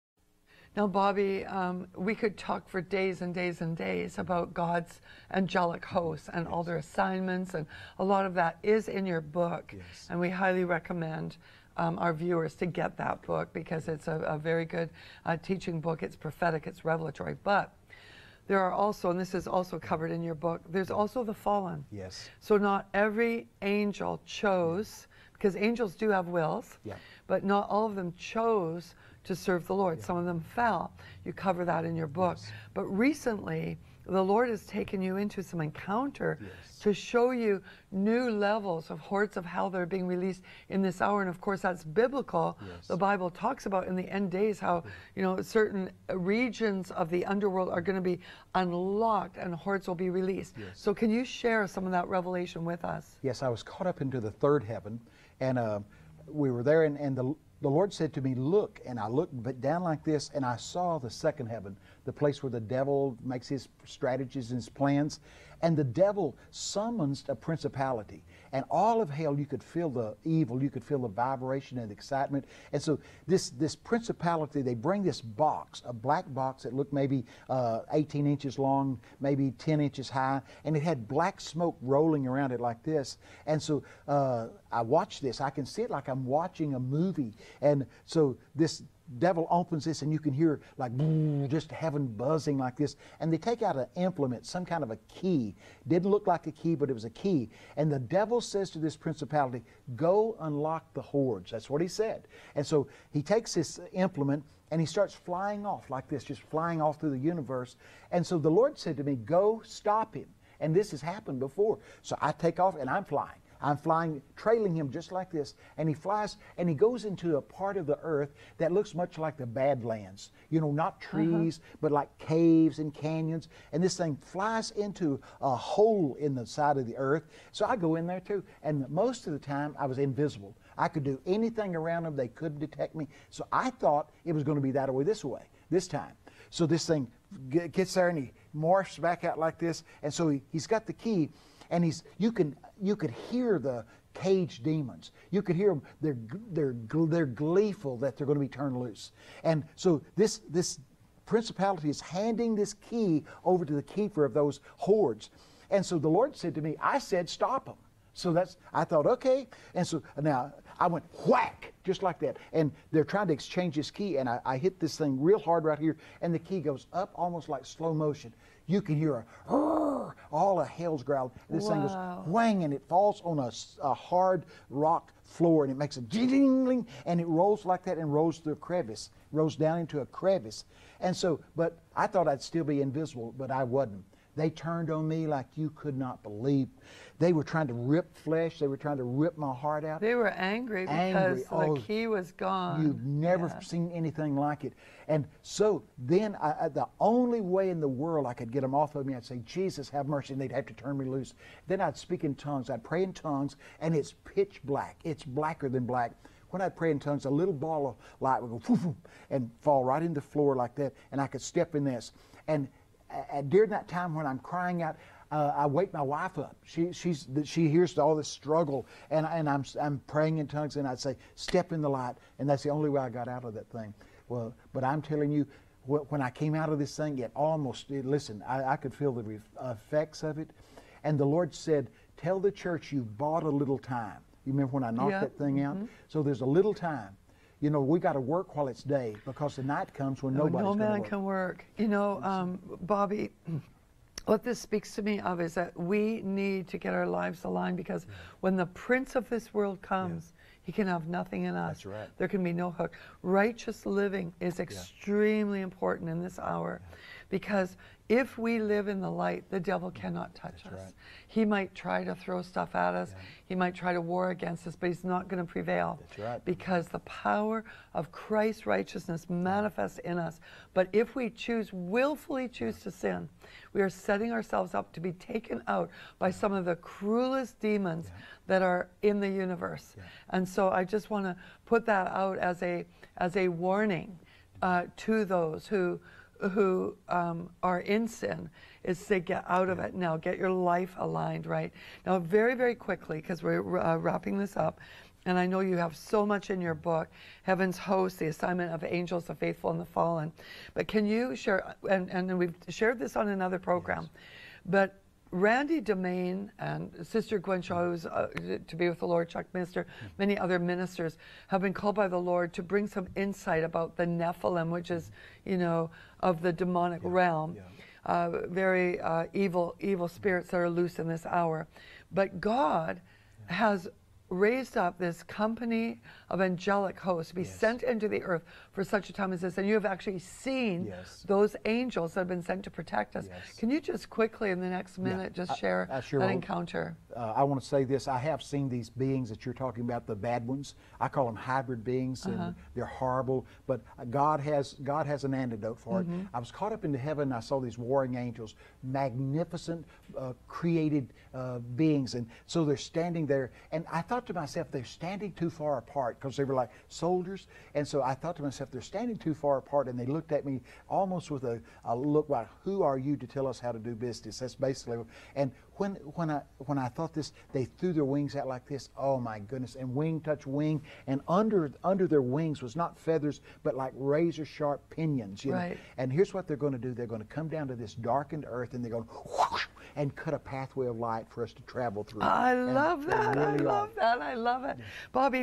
Hordes of Hell Are Trying to Come to Earth - Bobby Conner
Bobby Conner was taken to the third heaven and allowed to see the activity in the second heaven, where Satan's throne is. He tells his testimony in this 6 minute video.